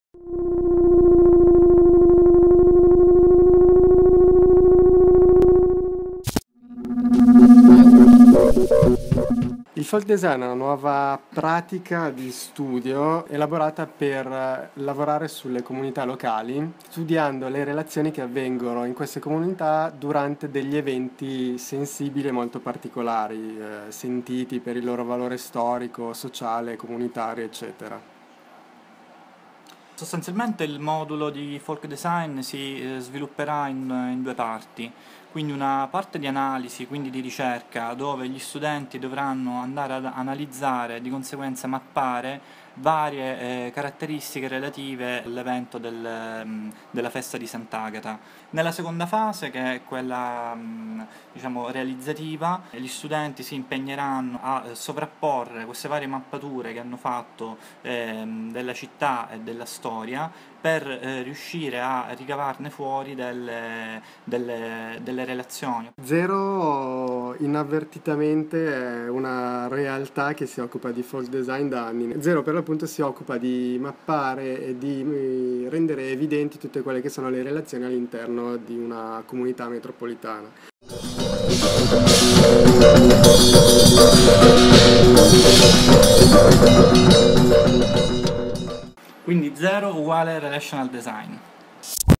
Folk Design: modelli narrativi partecipati
1-24 Giugno 2015 con Andrea Amichetti, Carlo Amico, Alessandro Busseni, in partnership con Zero.